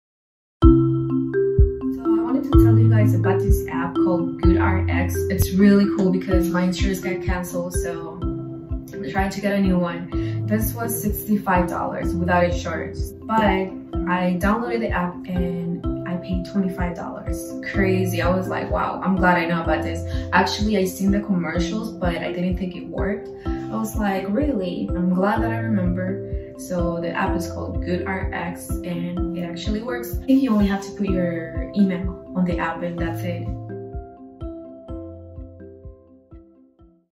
"Wow, I'm glad I know about this app." - Jacqueline M.
GoodRx really does work! No matter your situation, our coupons will help you save on your prescriptions.

If you have a GoodRx story to share, we'd love to hear it - just tag us at GoodRxHelps.